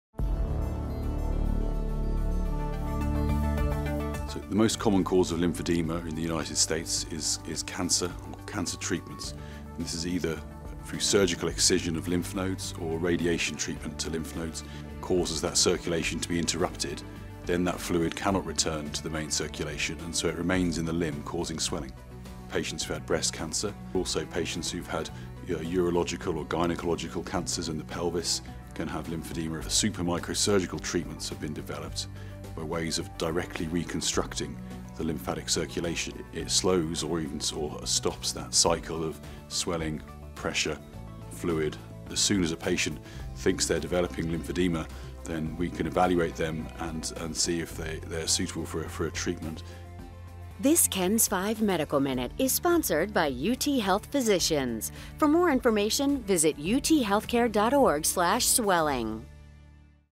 KENS 5 Medical Minute: Treating Lymphedema
Dr. Anton Fries, UT Health Physicians, reveals the causes of lymphedema, who is at risk and how new surgery is treating chronic swelling. This KENS 5 Medical Minute is a collaboration between UT Health San Antonio and KENS 5.